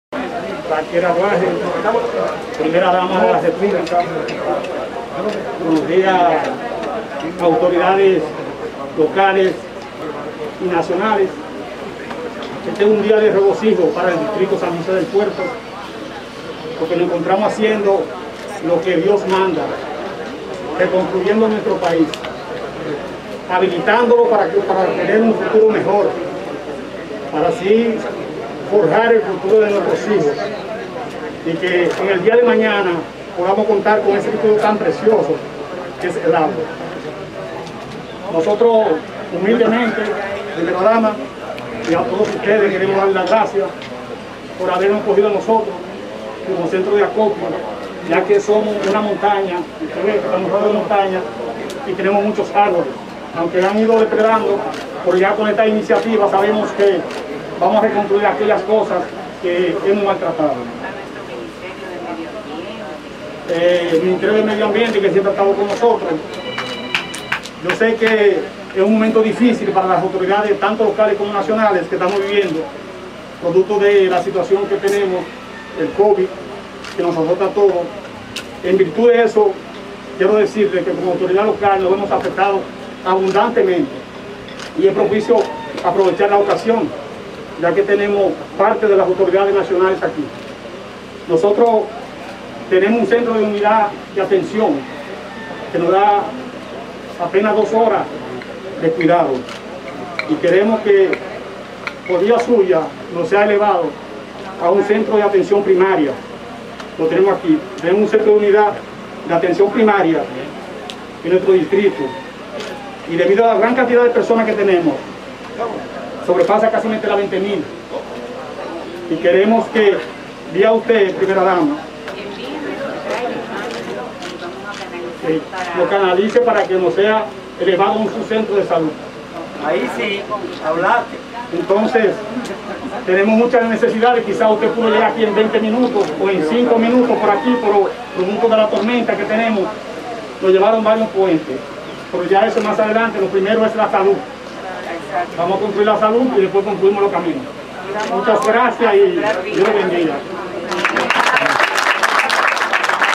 DELEGADO DE SAN JOSÉ DEL PUERTO PIDE A PRIMERA DAMA AMPLIACIÓN SERVICIOS HOSPITALARIOS .
El delegado municipal del distrito municipal de San José del Puerto de Villa Altagracia Caonabo Suriel, pronuncio un corto discurso en la jornada de reforestación patrocinada por la Primera Dama Raquel Arbaje y la viceministra de medioambiente Milagros De Camps en esa comunidad.
El referido discurso fue corto pero preciso. El delegado municipal del distrito municipal de San José del Puerto de Villa Altagracia pidió a la Primera Dama la ampliación de los servicios hospitalarios para esa comunidad.